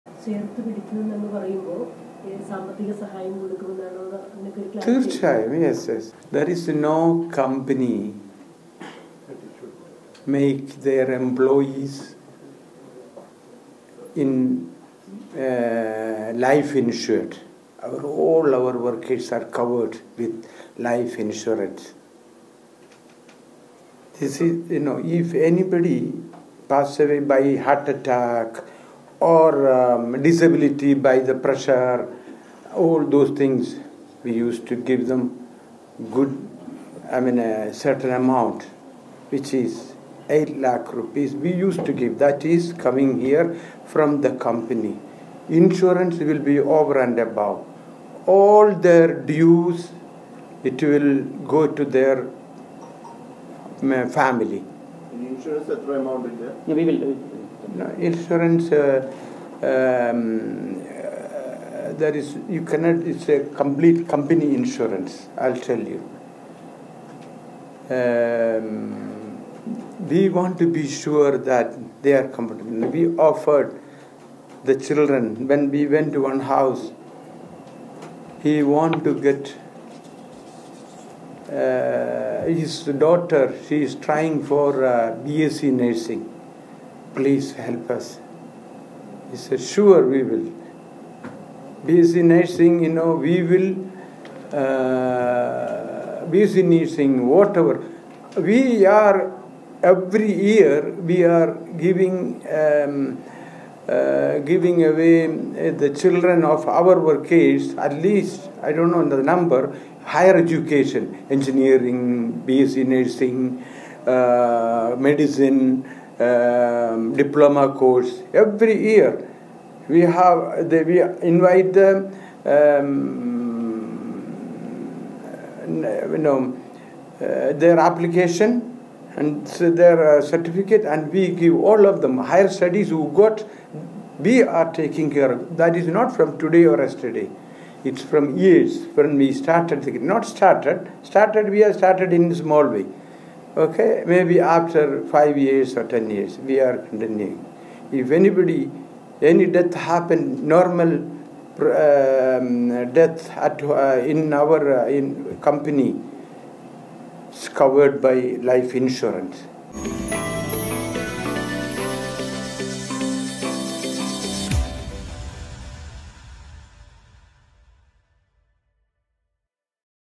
'തൊഴിലാളികളുടെ കുടുംബങ്ങളെ കമ്പനി ചേർത്ത് പിടിക്കും'
'എല്ലാവർക്കും ഇൻഷുറൻസ് പോളിസിയുണ്ട്, നാല് വർഷത്തെ ശമ്പളം ഇൻഷുറൻസ് തുകയായി കൊടുക്കും, അല്ലാതെ 8 ലക്ഷം രൂപ കമ്പനി നൽകും'